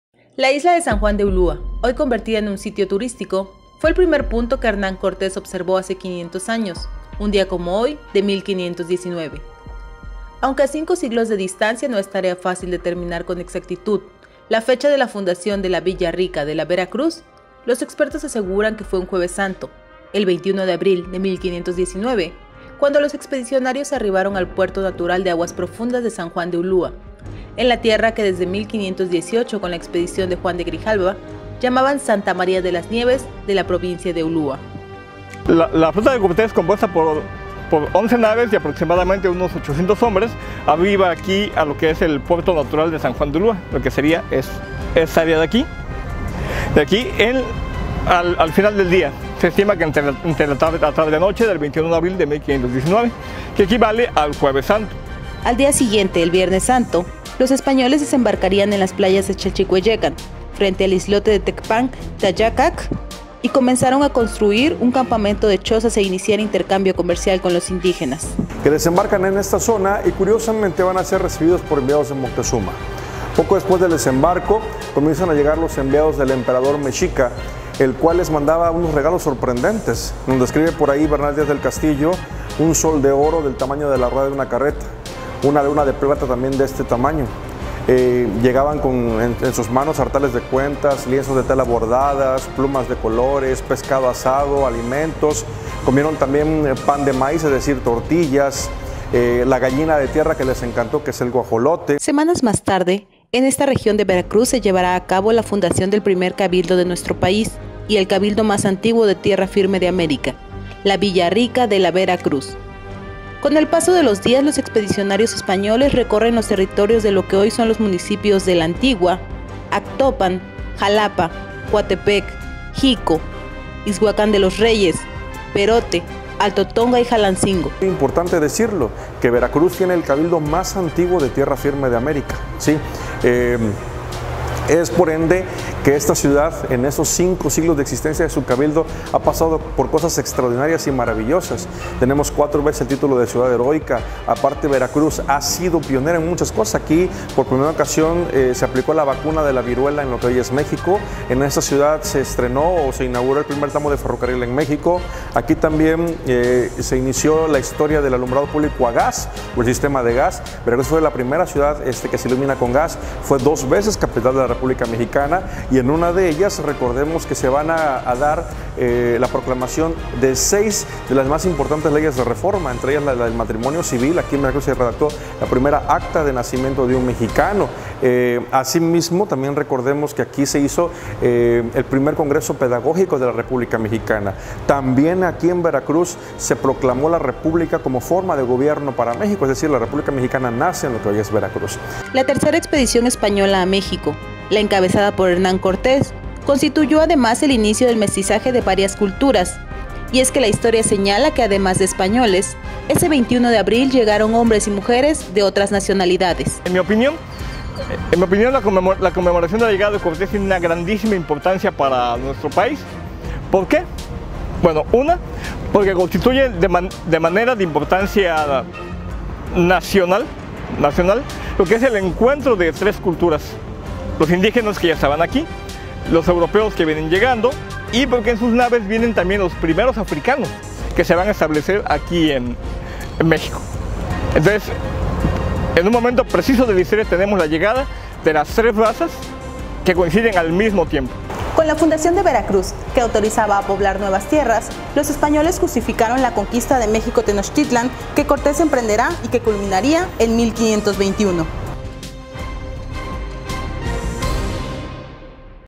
500 AÑOS VERACRUZ
Así fue el inicio del México mestizo con la llegada de Hernán Cortés a Veracruz